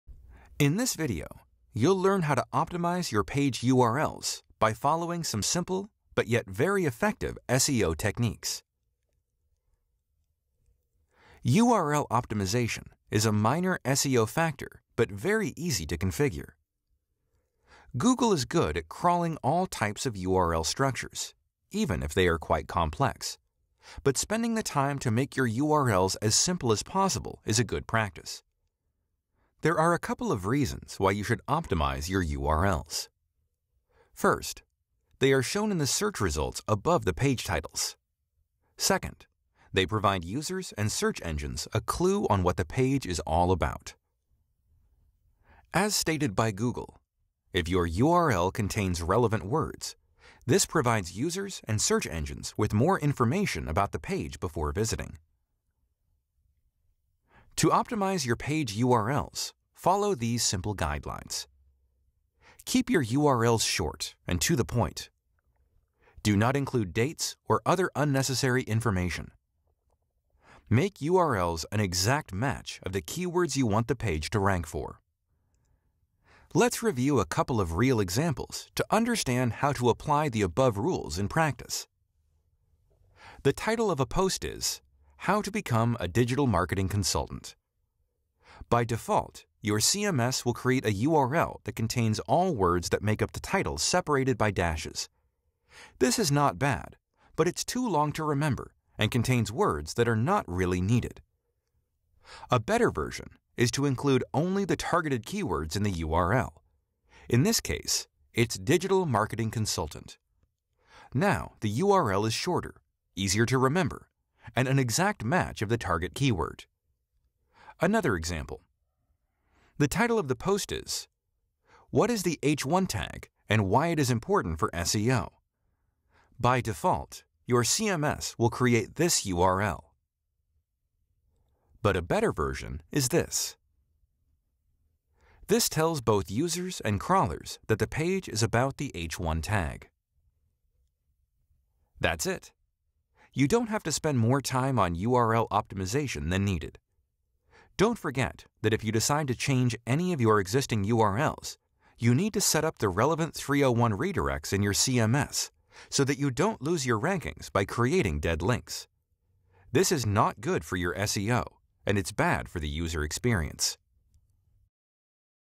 URL Optimization - How to Create the Perfect URL for SEO
👍 In this video, you’ll learn about Url Optimization and how to optimize your page URLs for SEO by following some simple but yet very effective SEO techniques.

By the end of this video, you'll know:

➜ What URL optimization is
➜ Why you should optimize your URLs
➜ How to create SEO friendly URLs

⏰TIMESTAMPS⏰
00:00 - Intro
00:55 - Page URL Optimization
01:20 - URL Optimization Examples